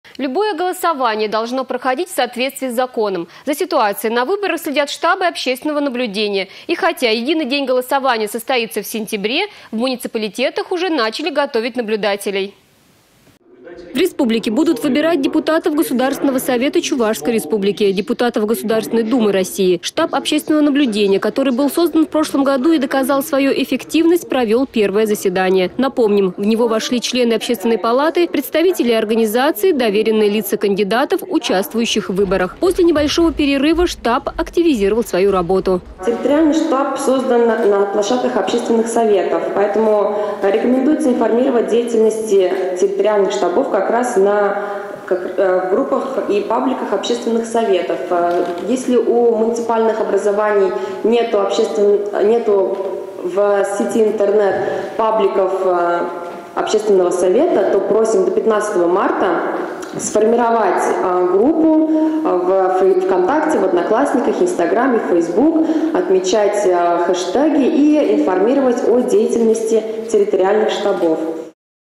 В муниципалитетах начали готовить наблюдателей на выборы
Любое голосование должно проходить  в соответствии с законом. За ситуацией на выборах следят штабы общественного наблюдения. И, хотя  единый  день  голосования состоится в сентябре, в муниципалитетах уже начали готовить наблюдателей.